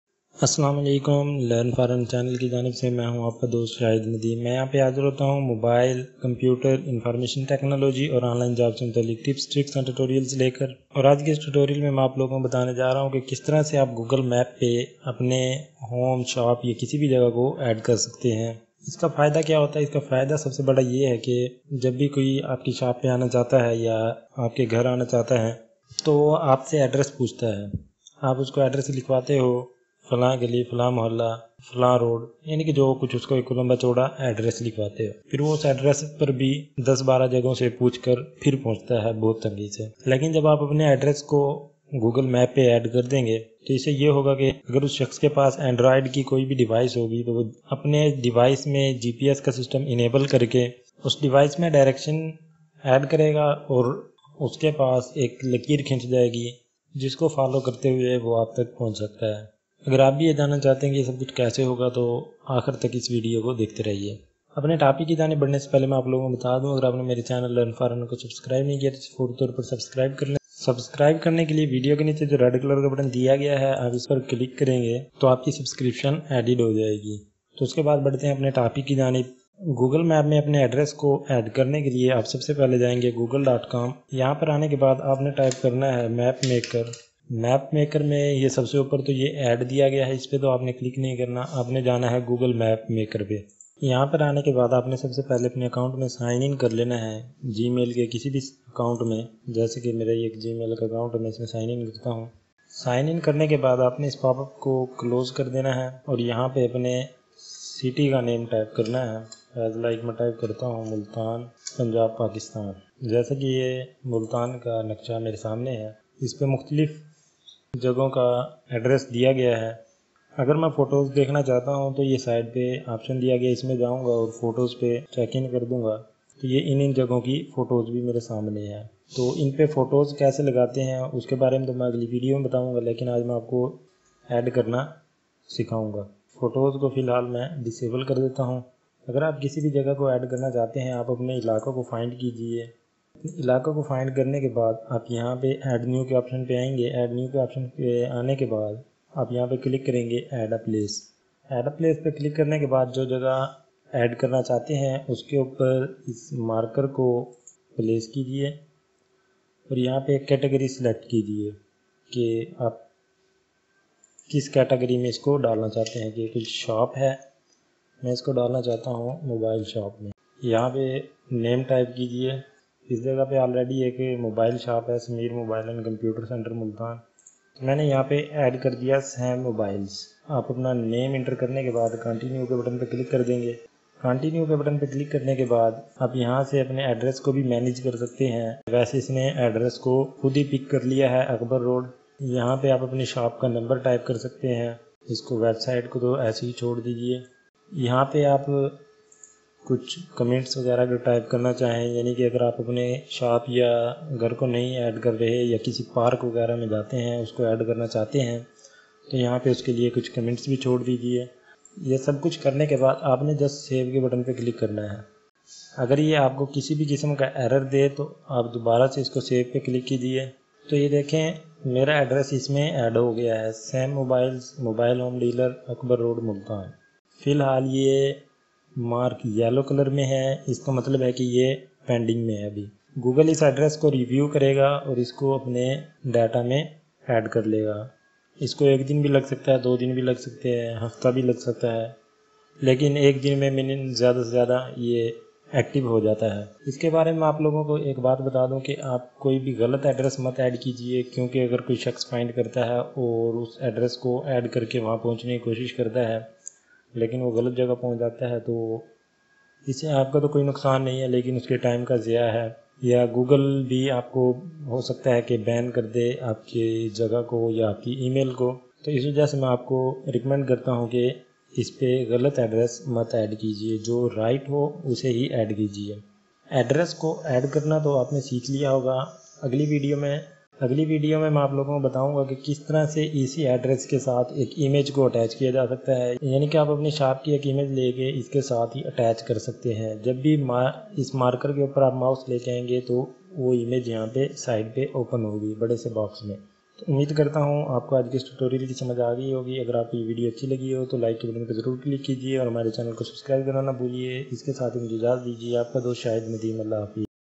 How to add Address in Google Map urdu hindi
In this VIdoe i'll describe you that how you can add you home,shop,college,canteen,cafe,club and other places in google map.
To know how to do all this Keep watching this video till end.